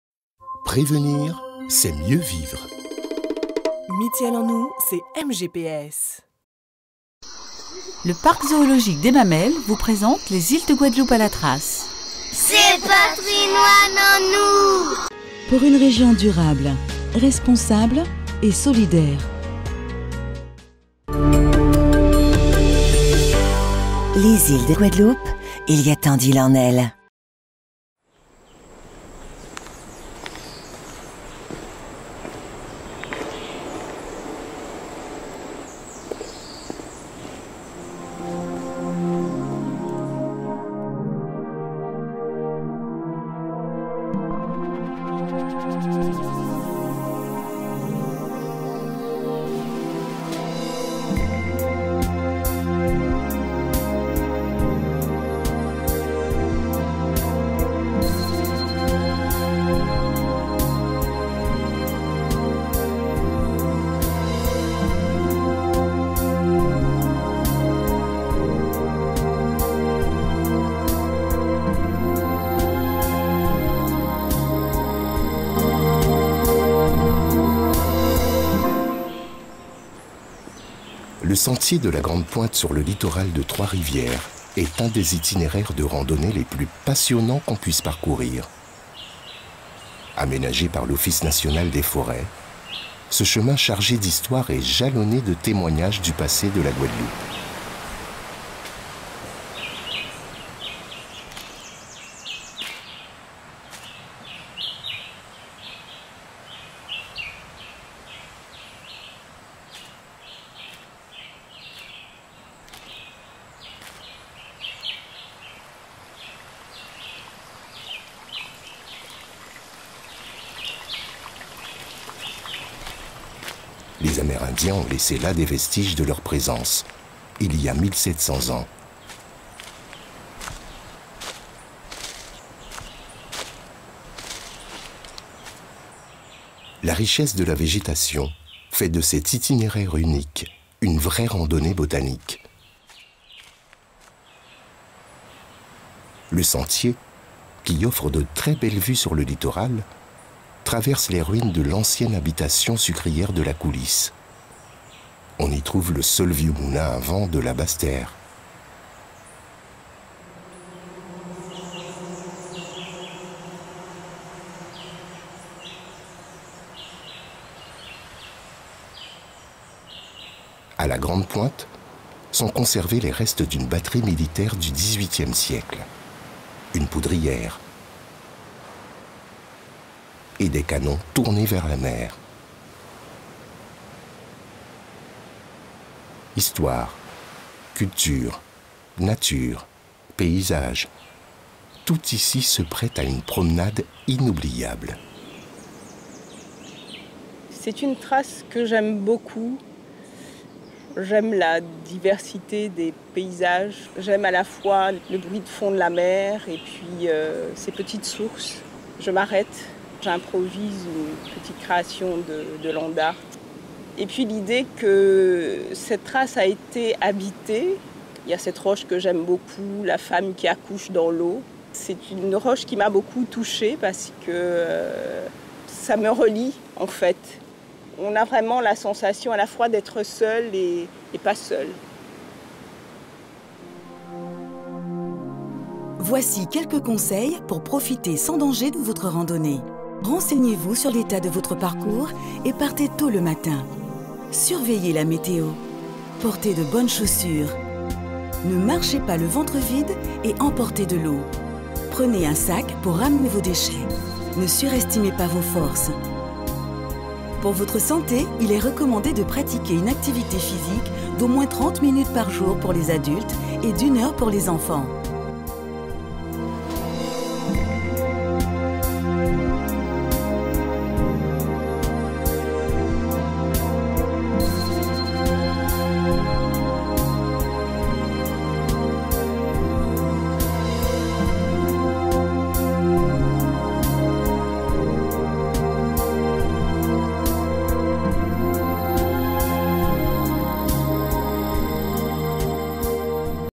Sur les traces Guy GABON au sentier de la grande pointe, Trois-Rivières
Pratiquez une activité physique tout en découvrant le patrimoine naturel des Iles de Guadeloupe.